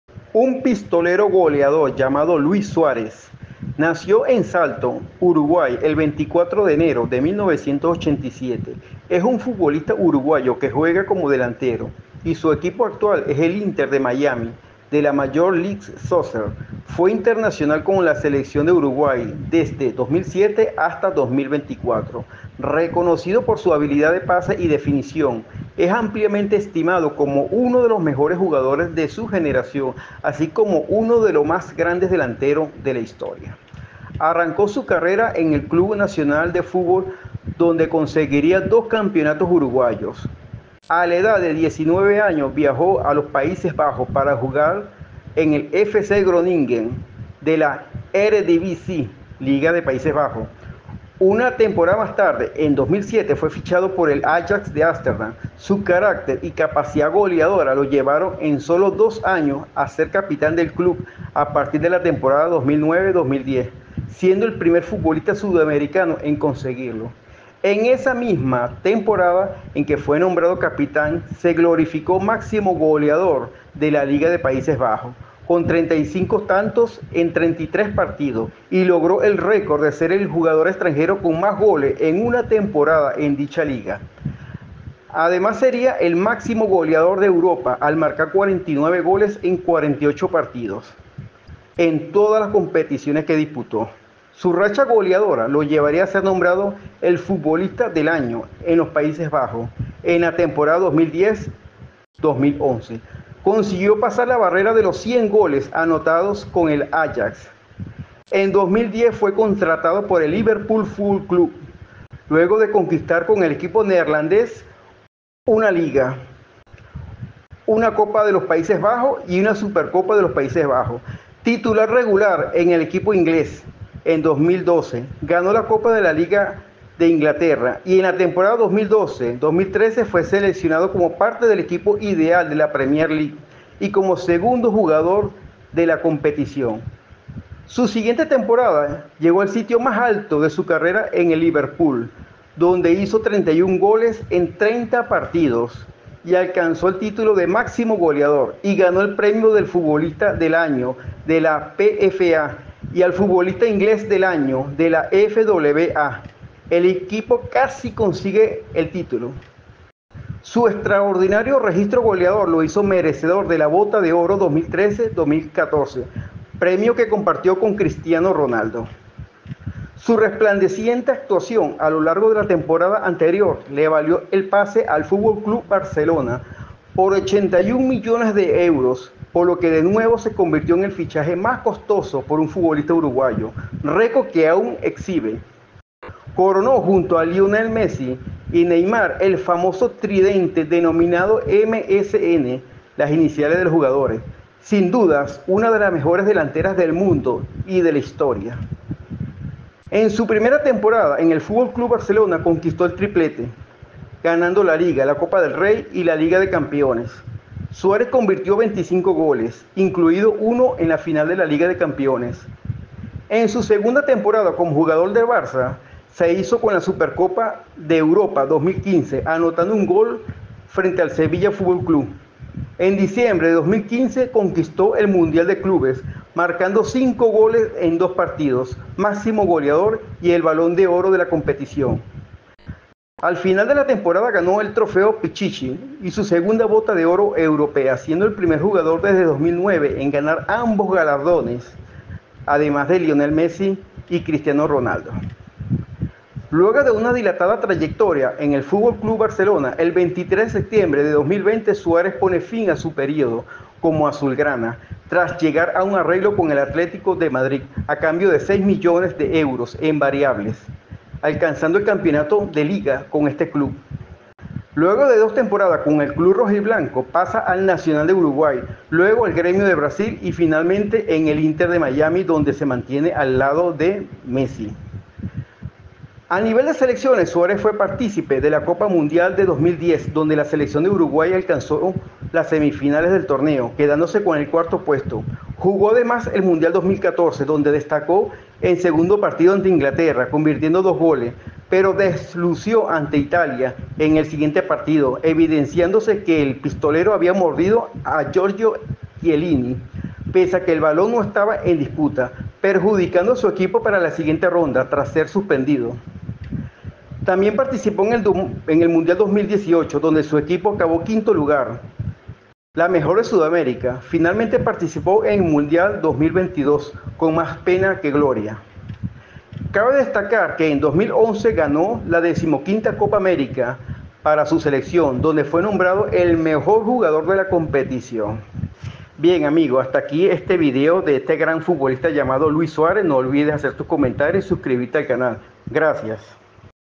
🇺🇾 Un Pistolero 🔫 goleador llamado | LUIS SUÁREZ
Luis Alberto Suárez Díaz es un futbolista uruguayo que juega como delantero y su equipo actual es el Inter Miami de la Major League Soccer. Fue internacional con la selección de Uruguay desde el 7 de febrero de 2007 hasta el 6 de septiembre de 2024

𝓜𝓾𝓬𝓱𝓪𝓼 𝓰𝓻𝓪𝓬𝓲𝓪𝓼
► ¡Sígueme!
► Instagram!   / refudizaje
► Twitter!   / refudizaje